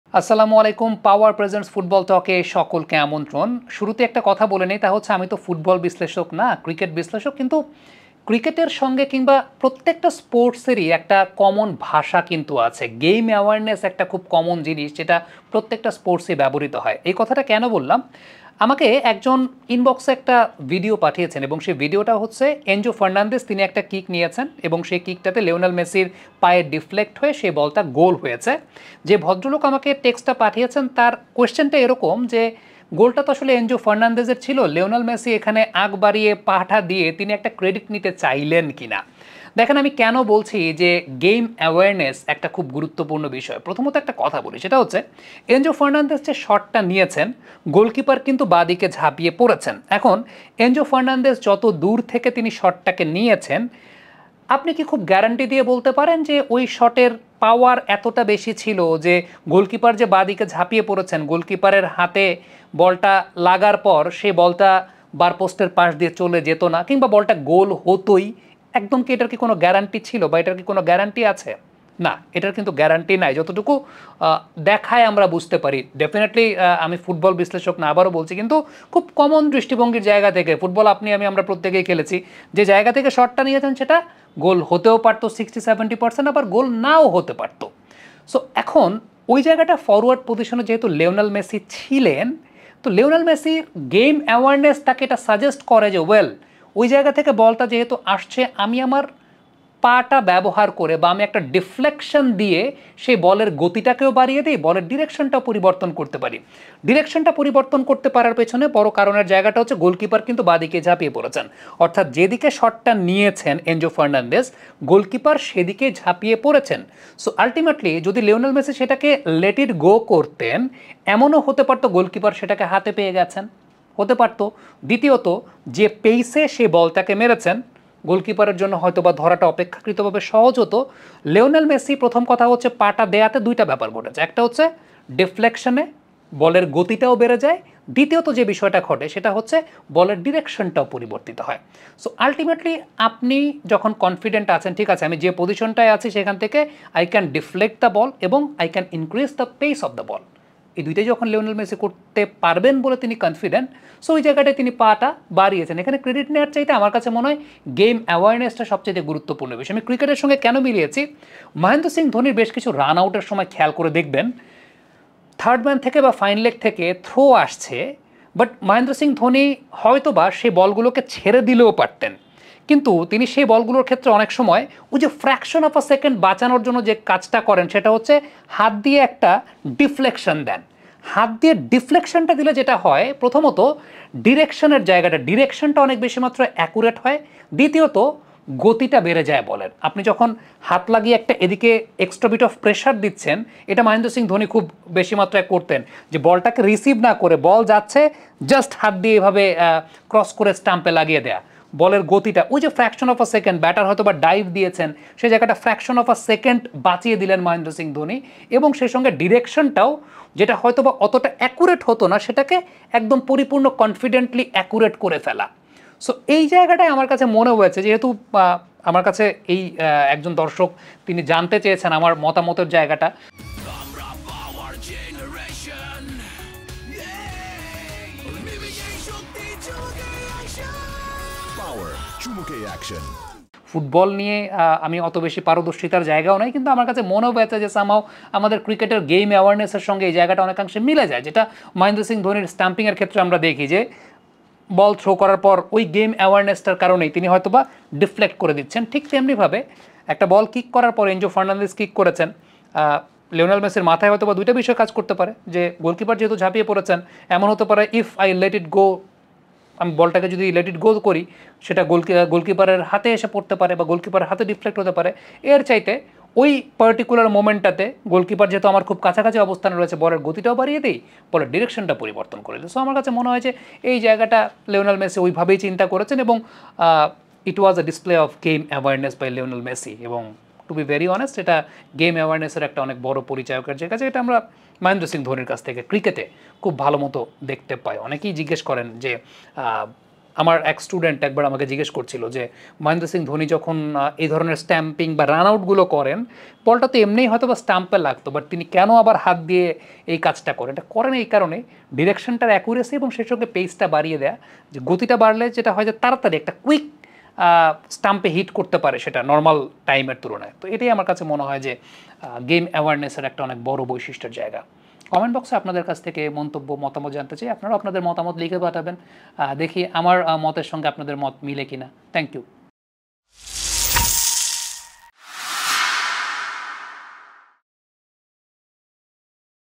মেসির সেমি'র গোলটা গেইম অ্যাওয়ারনেসের প্রমাণ, যেমনটা ধোনি করেন রান আউটে৷ || On Field 2024
𝐇𝐎𝐒𝐓
𝙎𝙮𝙚𝙙 𝘼𝙗𝙞𝙙 𝙃𝙪𝙨𝙨𝙖𝙞𝙣 𝙎𝙖𝙢𝙞
1st DIV. Cricketer and Cricket Analyst

𝐏𝐫𝐨𝐝𝐮𝐜𝐭𝐢𝐨𝐧
ON FIELD

𝐏𝐫𝐨𝐝𝐮𝐜𝐭𝐢𝐨𝐧 𝐓𝐞𝐚𝐦
𝐂𝐚𝐦𝐞𝐫𝐚𝐦𝐚𝐧       Shishir Sarker